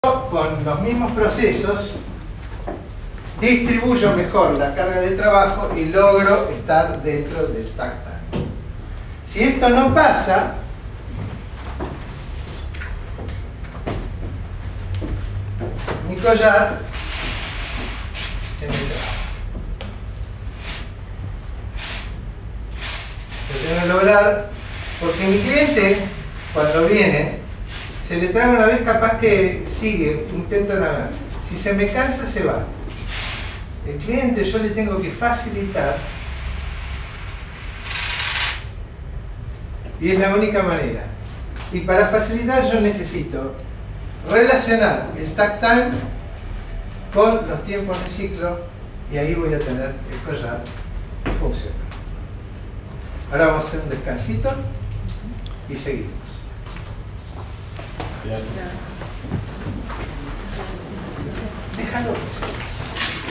Kaizen Club 14 agosto 2018 - 3
Seminario de Kaizen, Management Japonés y Lean Manufacturing – Docentes: Dr. José María Kokubu, Ing. Graciela Inés Ramírez y Lic. Leonardo Martín Rosso.